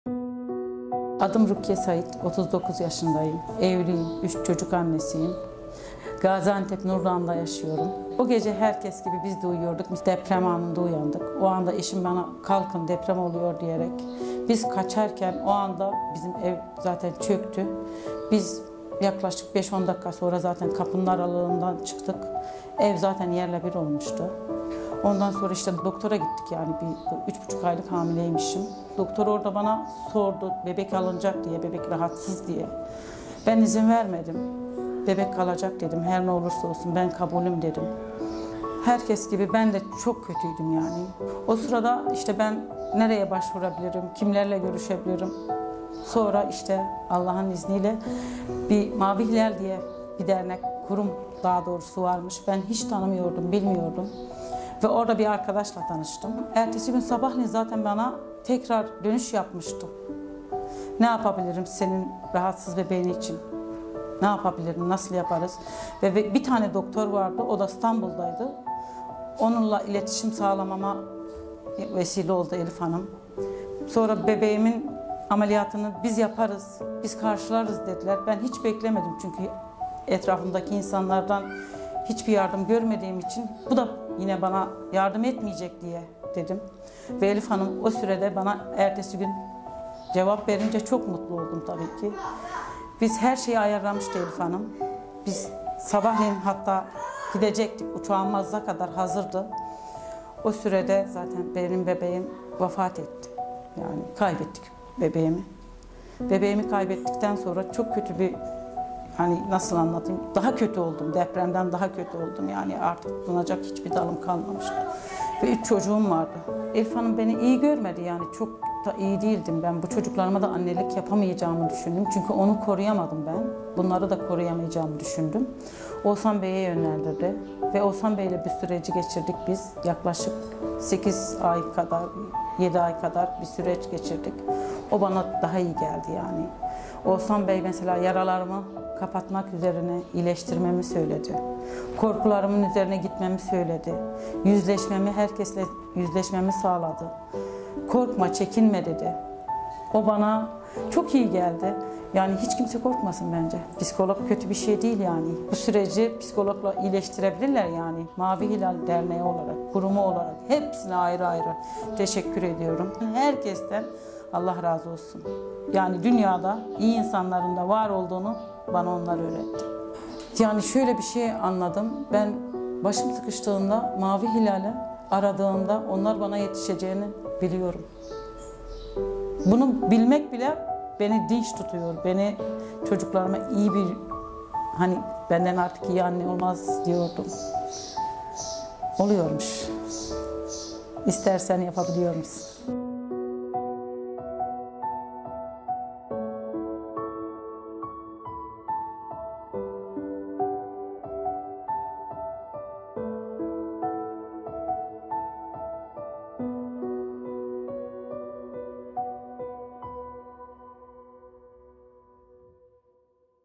Zorlukların Üstesinden Gelmek ve Yeniden Gülümsemek Mümkün
"Dünyada hala iyi insanların olduğunu bana Mavi Hilal öğretti." Depremin kalbi parçalayan acısıyla sınandık, ama hayatın yeniden yeşerebileceğini öğrendik. Rukiye Hanım'ın yaşadığı büyük kayıptan sonra, Mavi Hilal Vakfı'ndan aldığı destekle hayata tutunma mücadelesi, hepimize umut oldu. Onun hikayesi, zorlukların üstesinden gelmenin ve yeniden gülümsemenin mümkün olduğunu gösteriyor. ❤️

"IBC taught me that there are still good people in the world." We were tested by the heartbreaking pain of the earthquake, but we learnt that life can flourish again. After the great loss Rukiye suffered, her struggle to hold on to life with the support she received from IBC gave hope to all of us. Her story shows that it is possible to overcome difficulties and smile again. ❤️